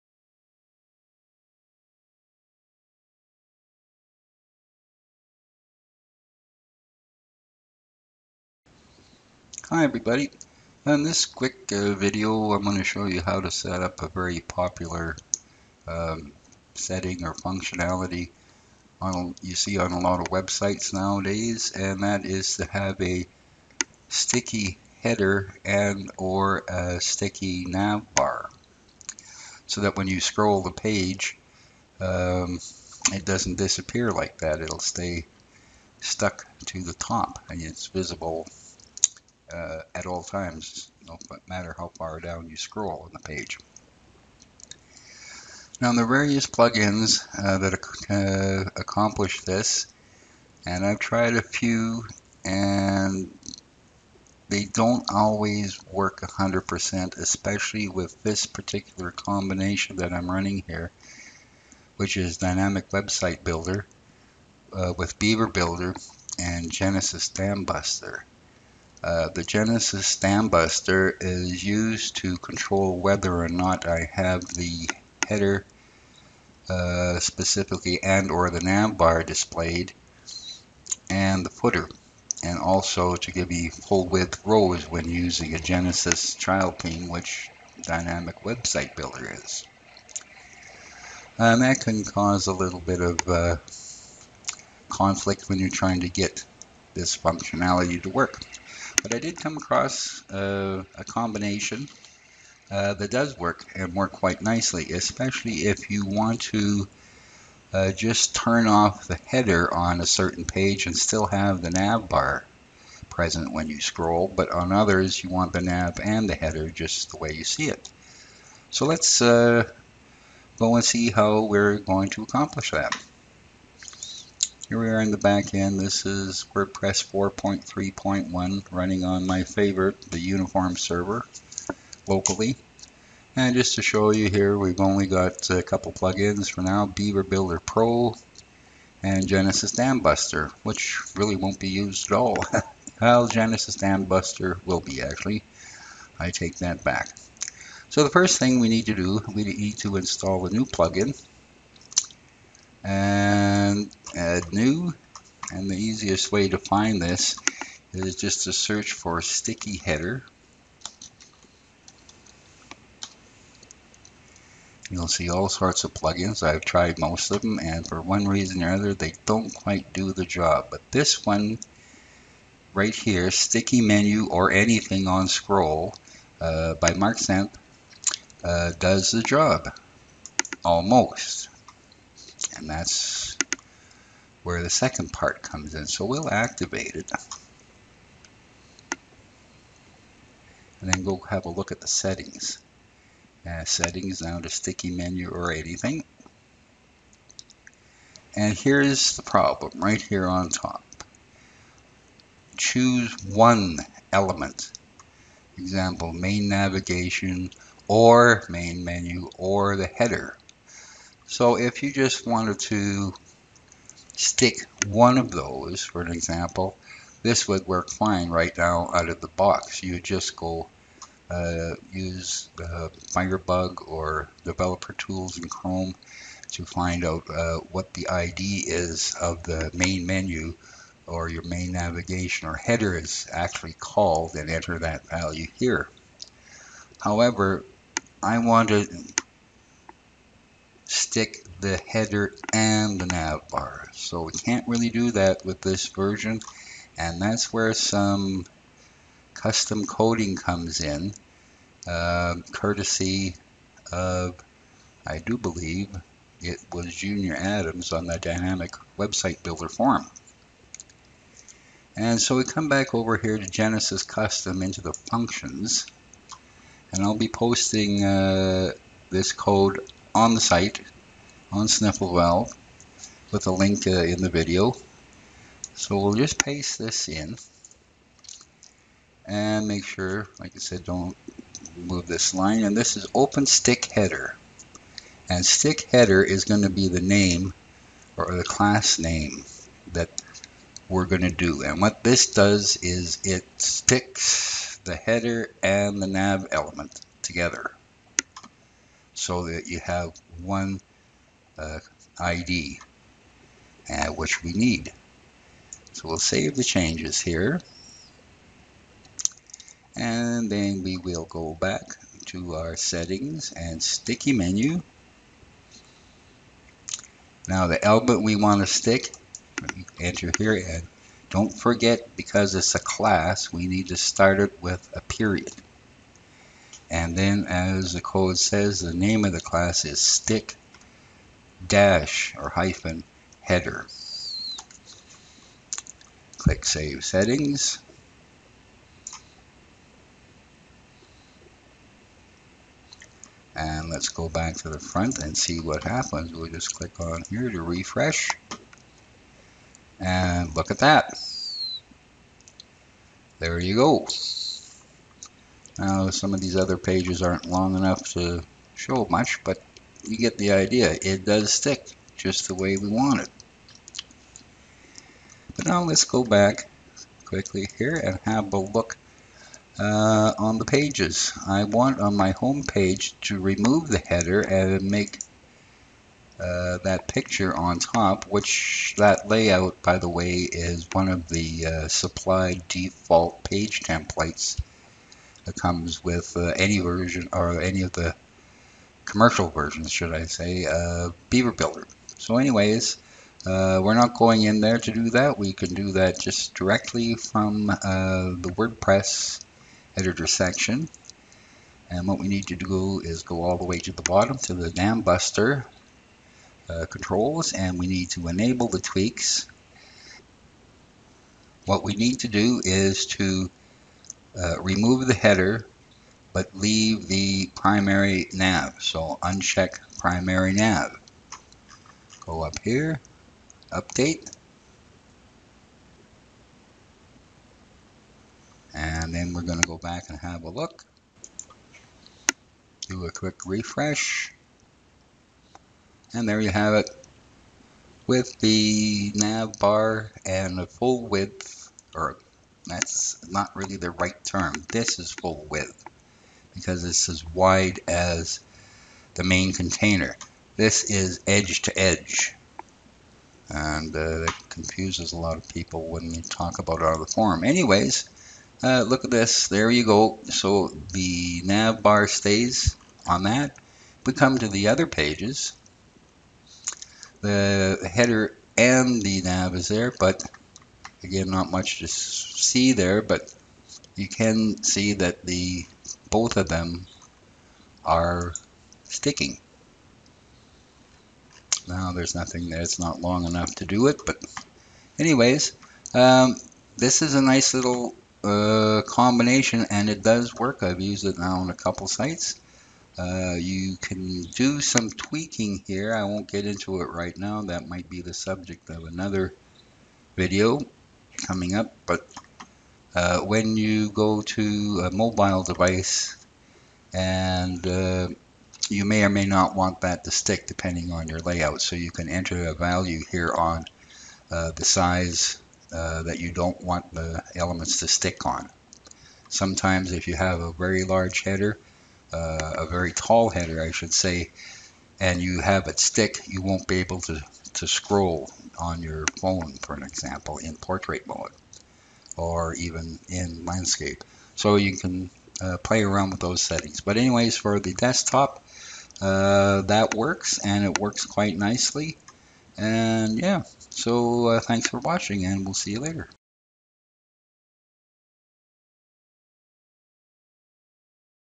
Dynamik Website Builder + Genesis Dambuster - Sticky Header and / or Menu Bar
How to set up a sticky header and or menu bar using a free plugin with a bit of custom code. No worries, code supplied here: